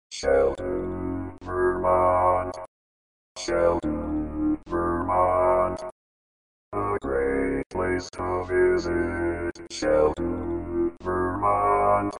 How to Say or Pronounce USA Cities — Sheldon, Vermont
This video shows you how to say or pronounce Sheldon, Vermont.

A computer said Sheldon, Vermont. How would you say Sheldon, Vermont?